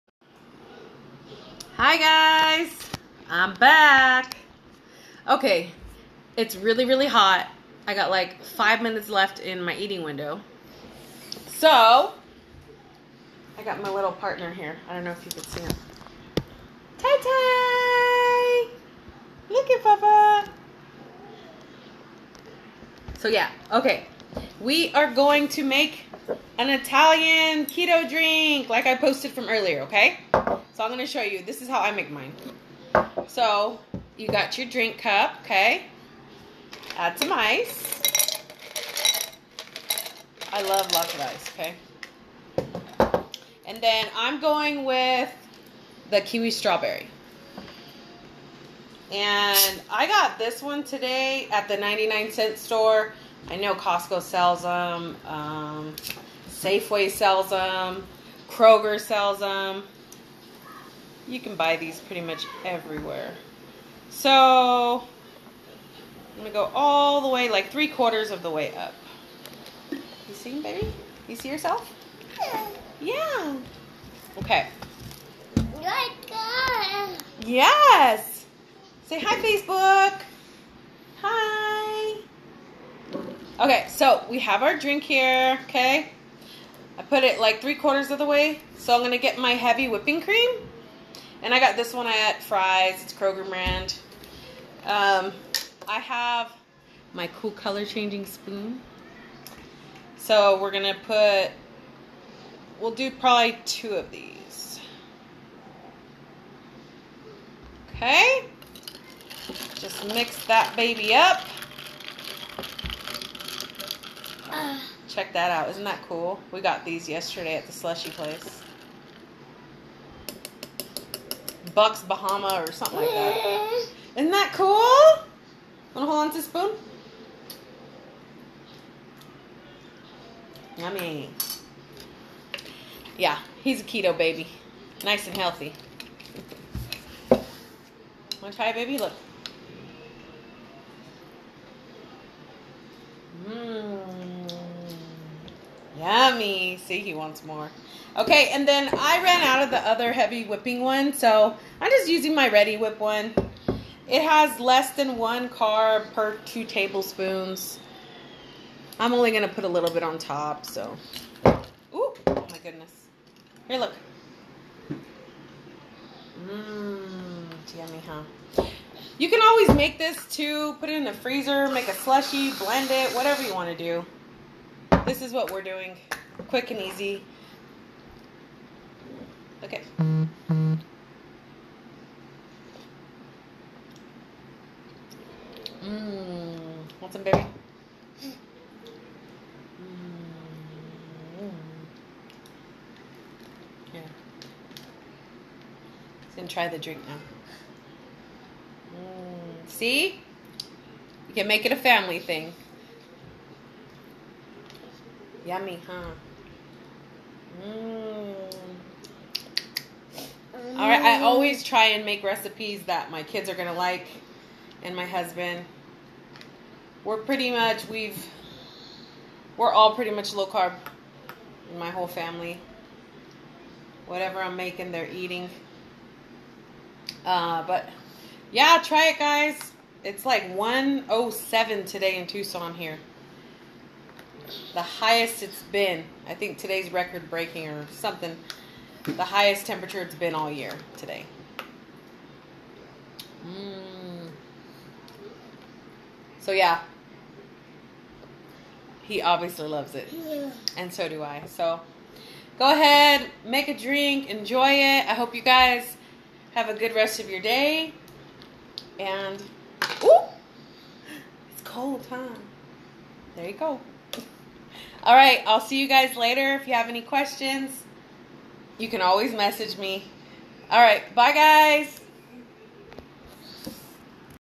Keto italian ice
Cool off with this  refreshing meto Italian ice drink
Sparkling ice sparkling water (flavor of your choice)
2tbsp heavy whipping cream
Can of whipped heavy cream and strawberries to top for garnish
And ice!